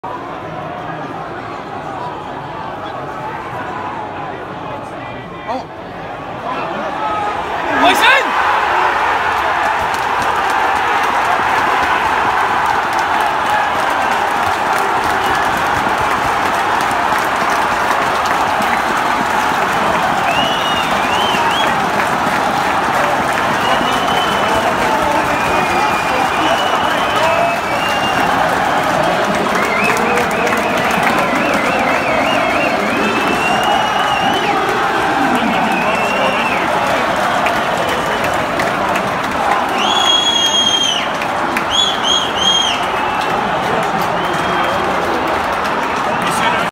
Dunfermline 2 Falkirk 0 - the Pars take the lead
Dunfermline open the scoring against Falkirk in the Kincardine Bridge derby to send the home fans wild. The top of the table Scottish League 1 clash was watched by over 9500 spectators at East End Park, a tremendous effort for a 3rd tier match on a Tuesday night.